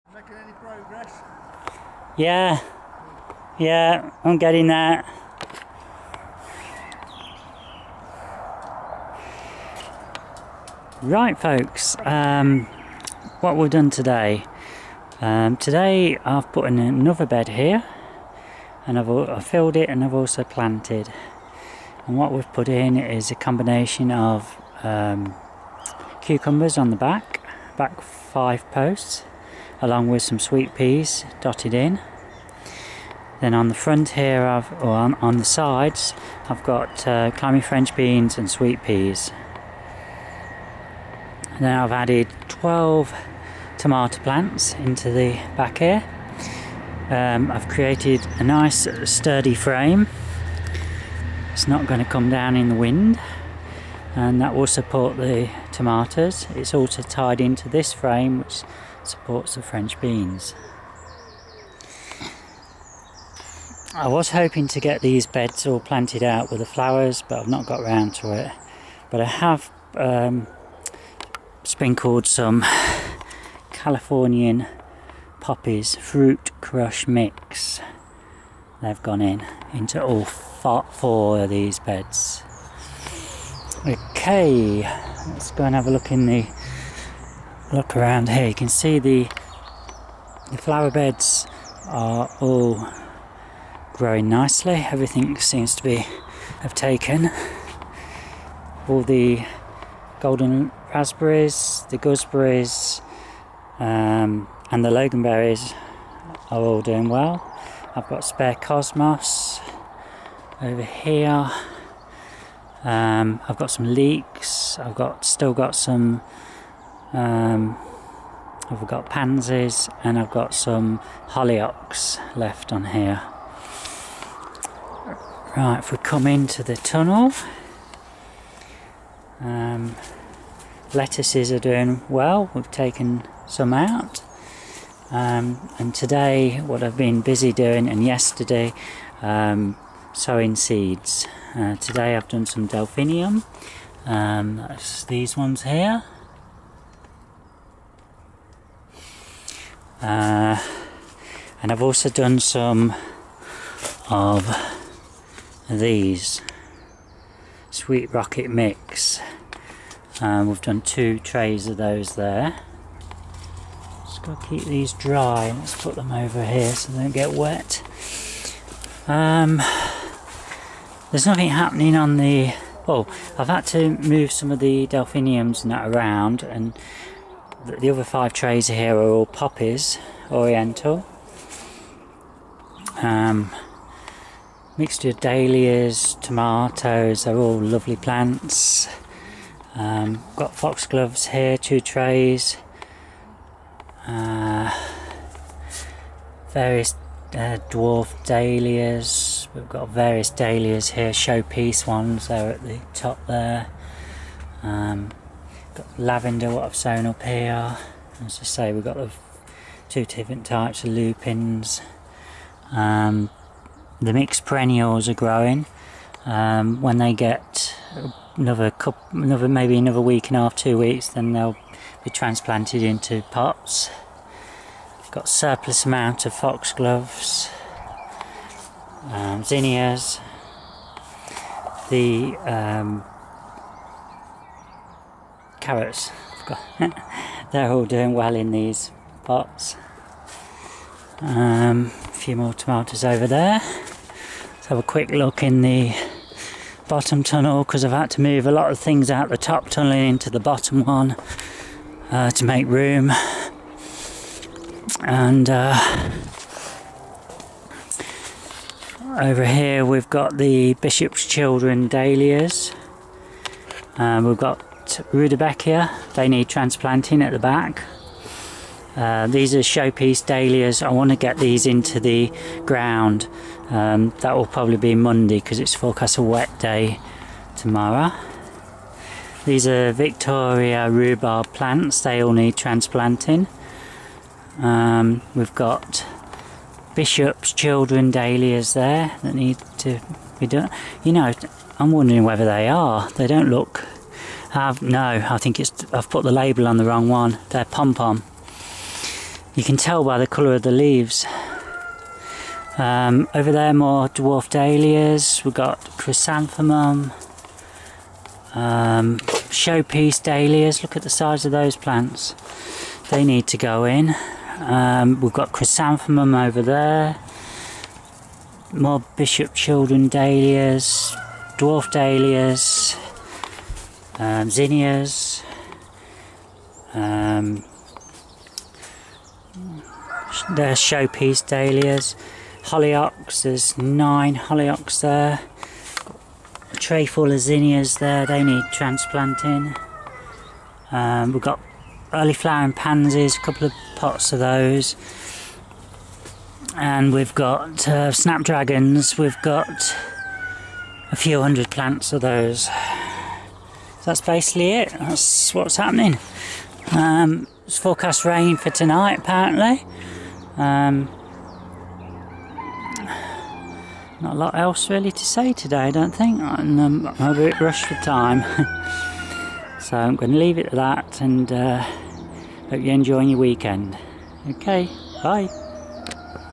Julie's Allotment - New Raised Bed and Seeds - 30th May 2015
Another bed introduced today, tomatoes, cucumbers and left over climbing French Beans planted into 2 raised beds.  Also sowed some more seeds for next years benefit.